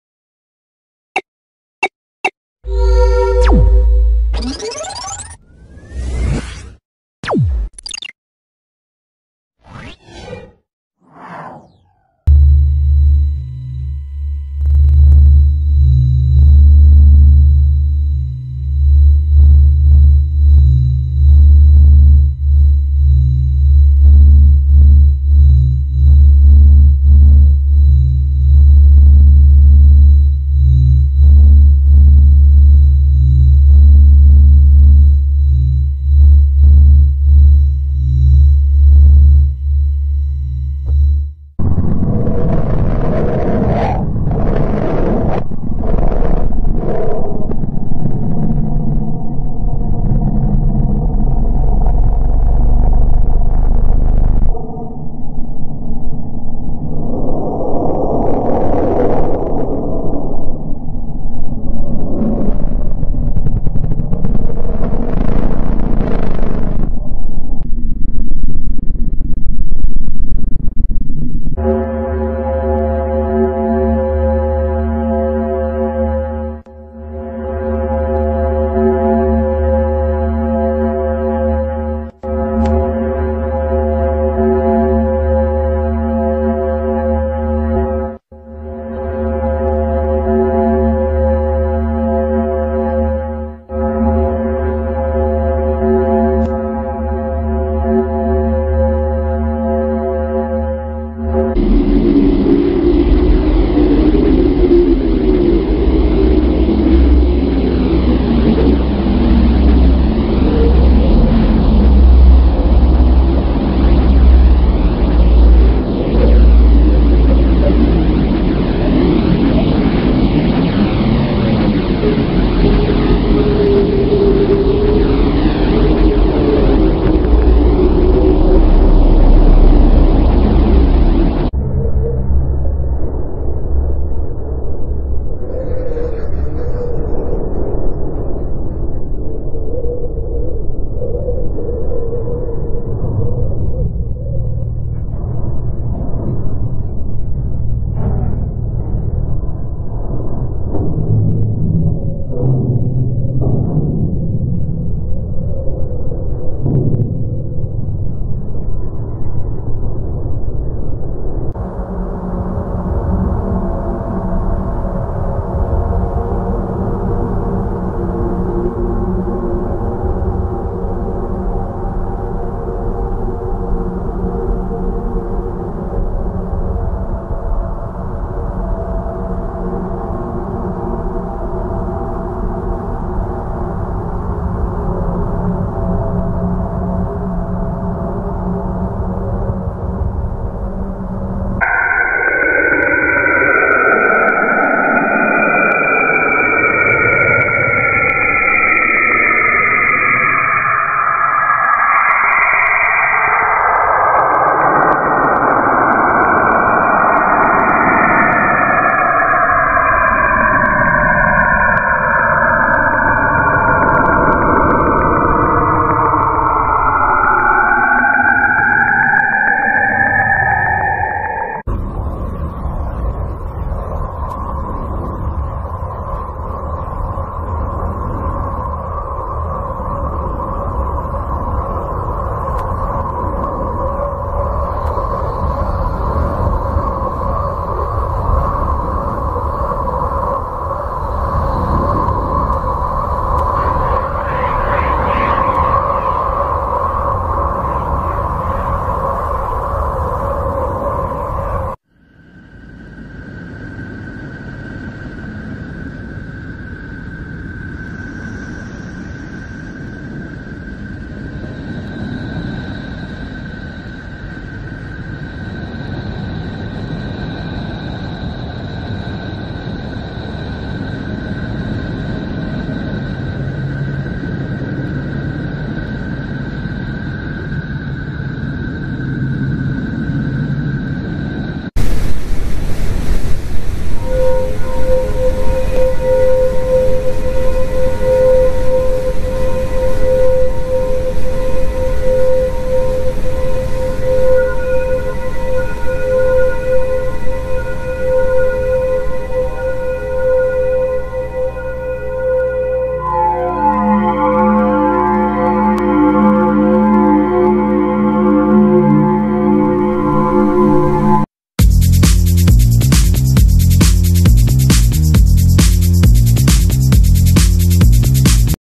All Planet sounds of our Solar System | Space sounds
NASA Space Sounds: what happens when spacecraft are used to record radio emissions from planetary environments, which are then converted to sound waves.

Here’s something you don’t hear everyday. This video presents NASA Space Sounds, giving you an idea of what different objects in our solar system might sound like, if you could hear them. More specifically, it’s a translation of electromagnetic vibrations from various planetary environments into sound.
So what are these sounds again? Some spacecraft have instruments capable of capturing radio emissions. What you’re hearing are the result of scientists’ conversion of these radio emissions to sound waves. Instruments on NASA’s Voyager, INJUN 1, ISEE 1 and HAWKEYE space probes were used to record the vibrations of different objects in our solar system.